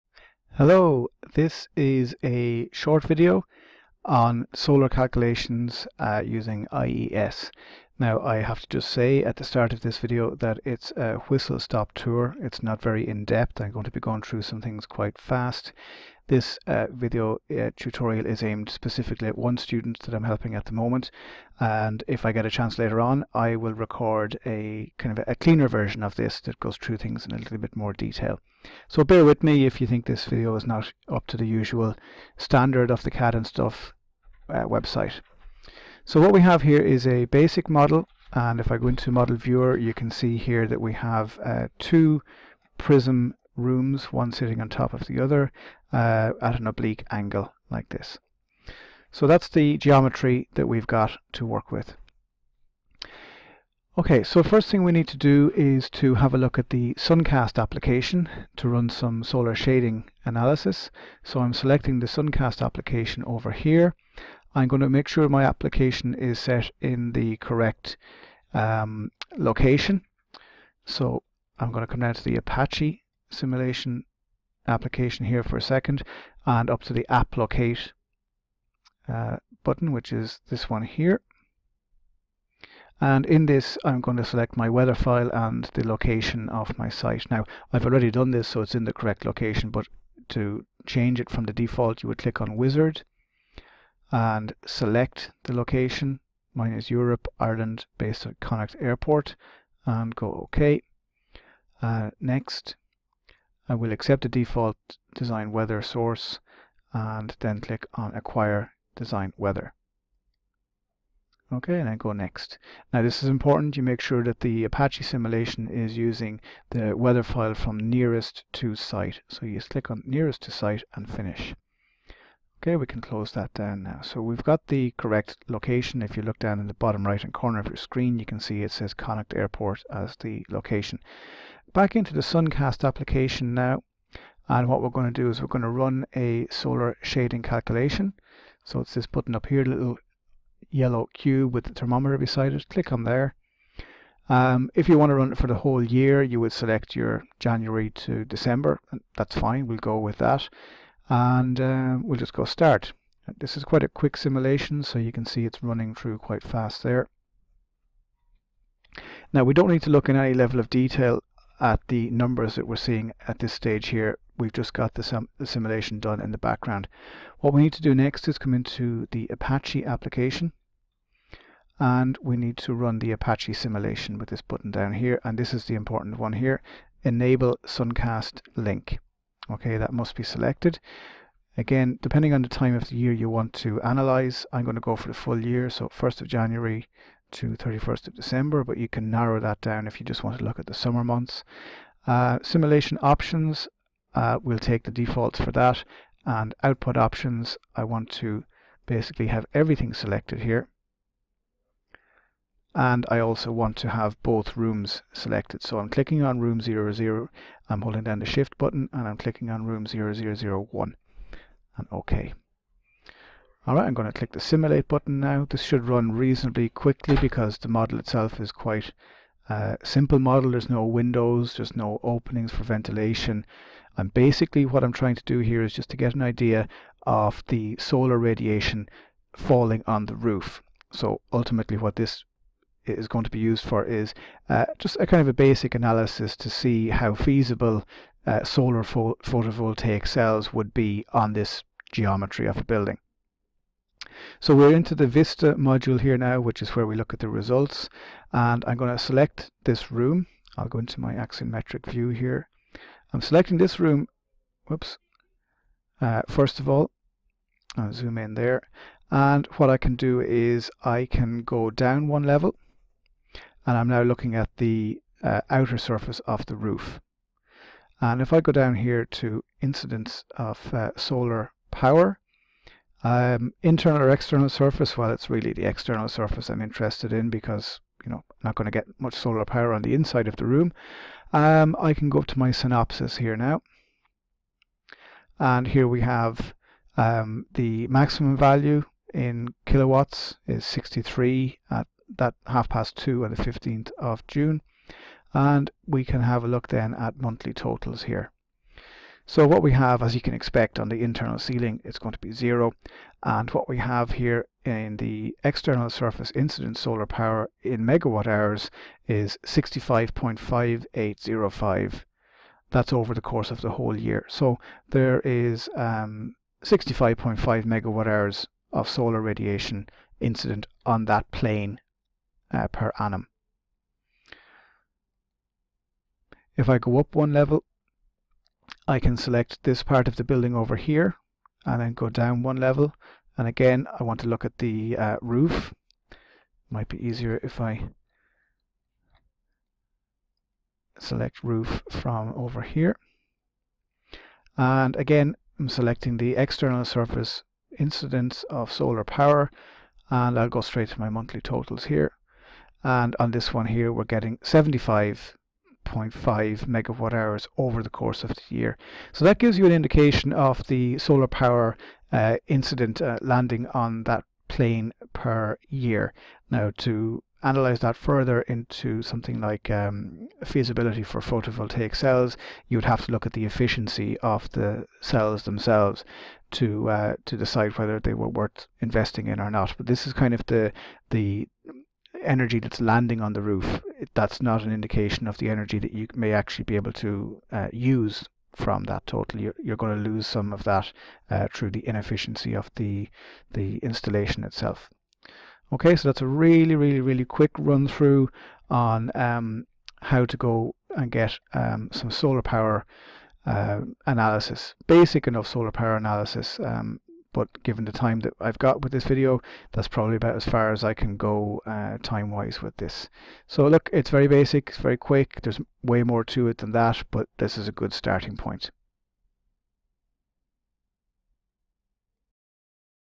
Basic Solar Analysis in IESVE
This shows a very quick tutorial through how to carry out a solar shading and solar power analysis on a simple building in IESVE.